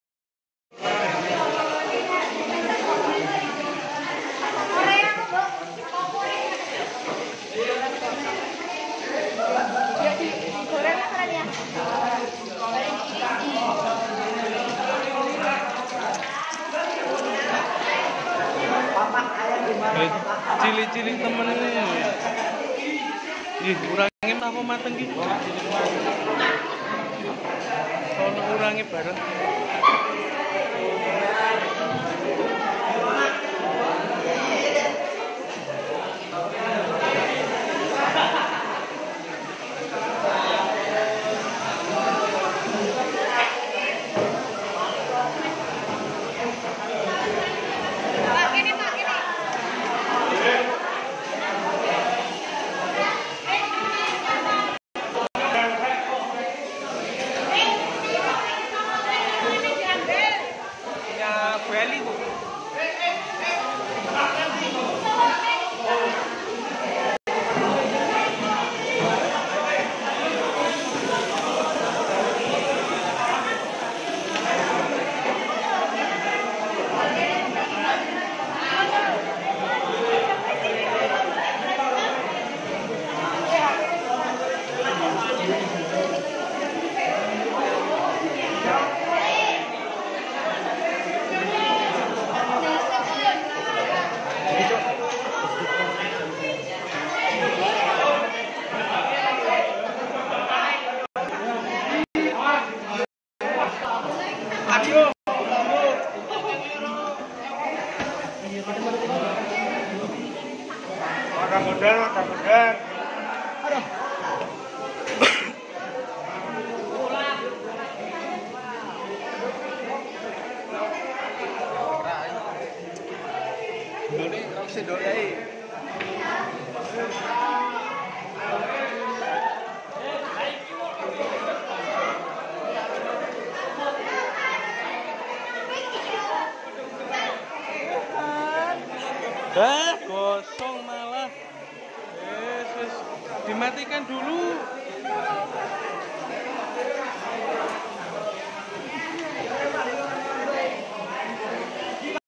Kegiatan P5 di SMK, Lomba Masak Enak
Kegiatan P5 di SMK, Lomba Masak Enak. Kegiatan ini merupakan kegiatan akhir proyek p5 yaitu lomba masak antar kelas di SMK Negeri 8 Purworejo.